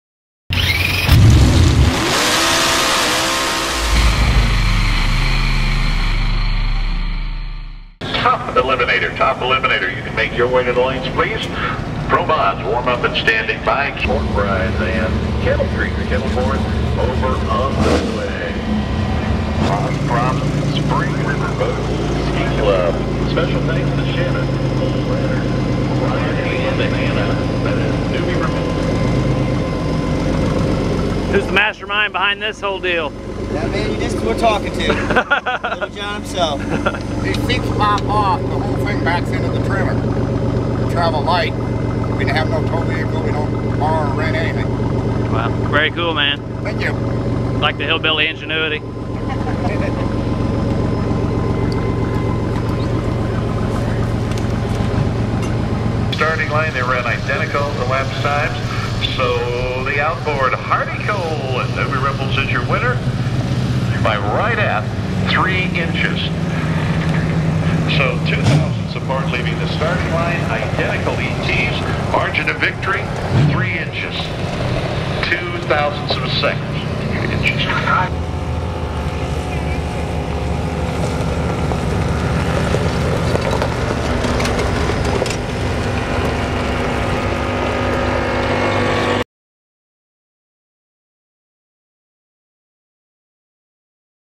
CRAZY Half Car Drag Boat Hauler
We love strange and unique home engineered vehicles , and this custom half car drag boat hauler is no exception. It actually has hydraulics underneath to extend the boat into the water and pull the front of the boat into the car when it's in the trailer.